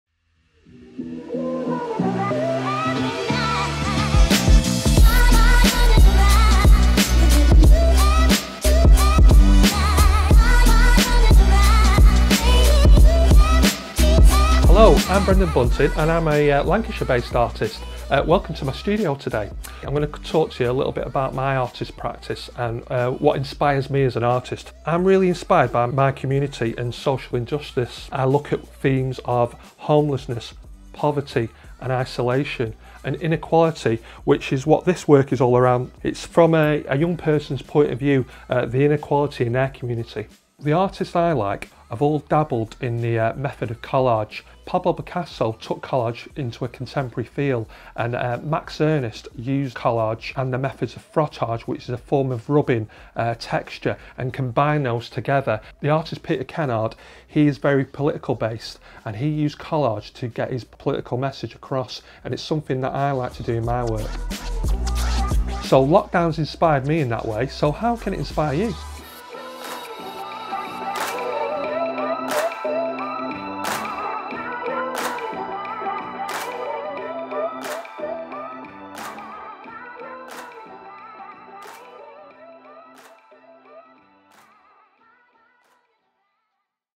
Brendan Bunting: Collage and Explore
This is the fourth video in Brendan Bunting's commission for The Living City.

In this video Lancashire based artist Brendan discusses how the community inspires him to create art and collage. He touches on some famous artists including Picasso and how they used collage in their work.

Over the course of these videos Brendan will introduce you to the process of frottage (creative rubbings) in different environments as well as how to exhibit your work.